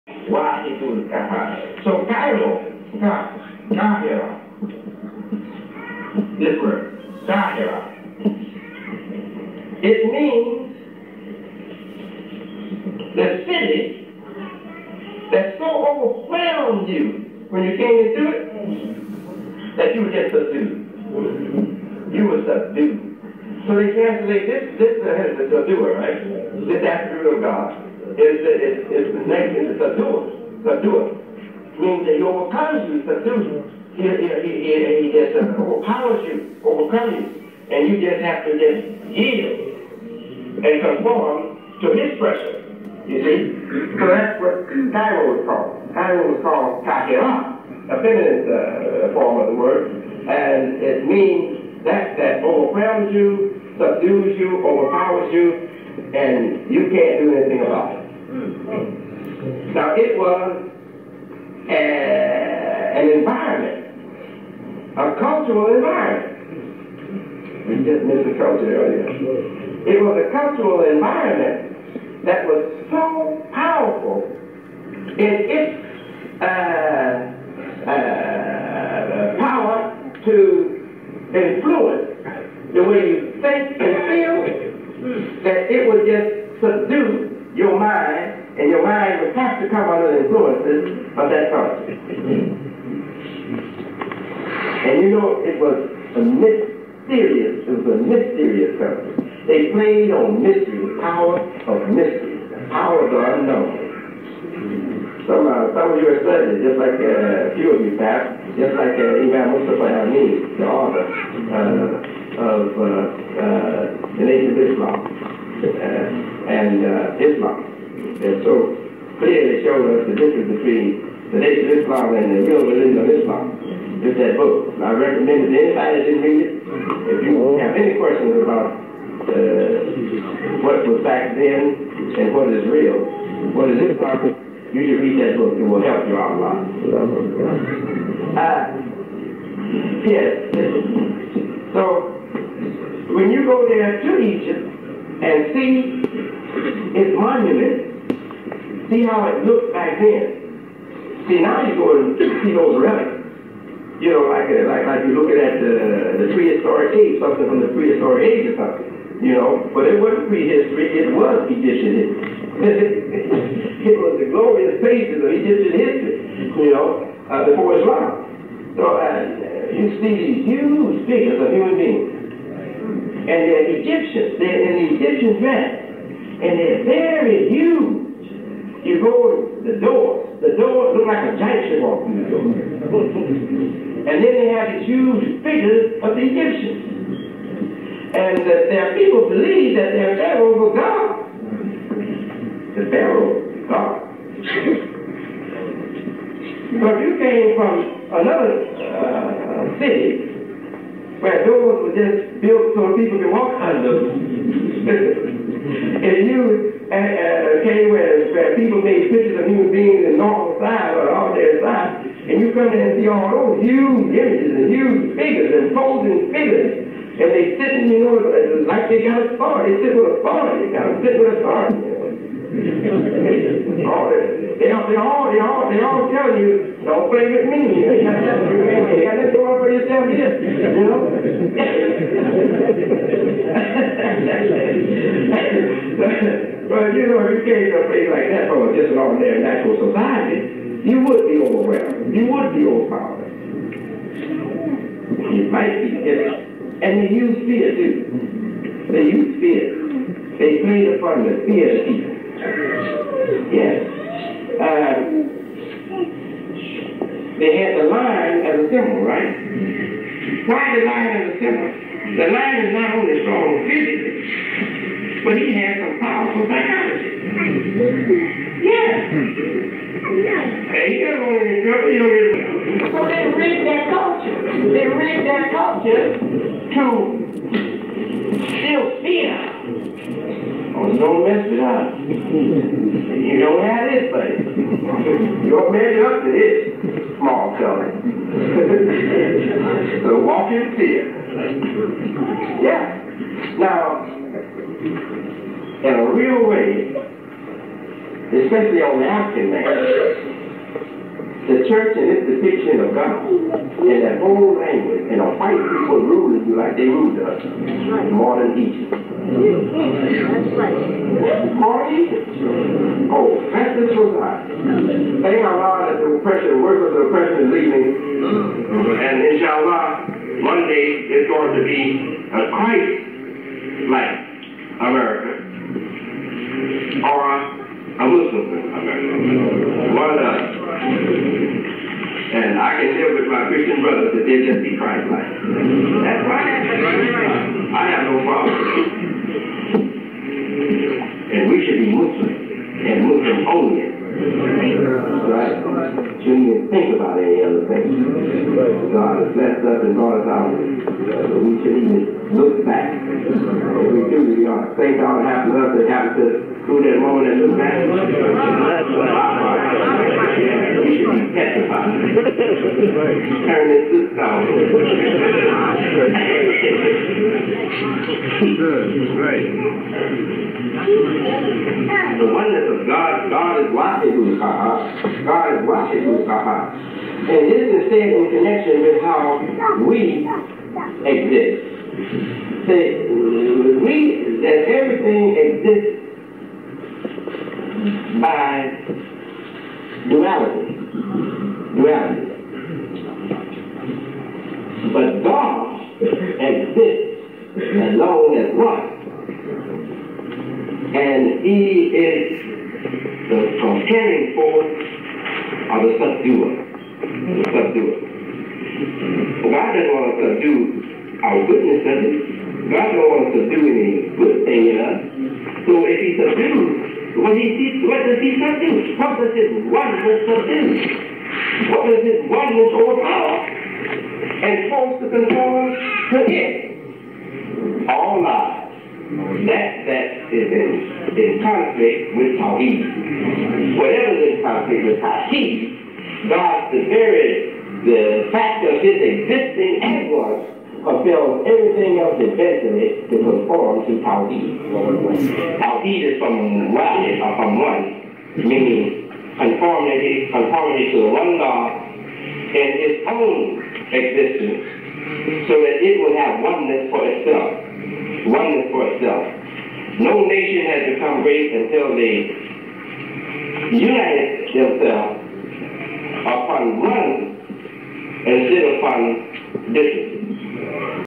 Imam W.Deen Mohammed:Allah, The One and Only, Al- Qahhar, The Subduer, (Qur'an 13:16).
Imam W.Deen Mohammed at Masjid Muhammad located in Newark,NJ. November 4,1995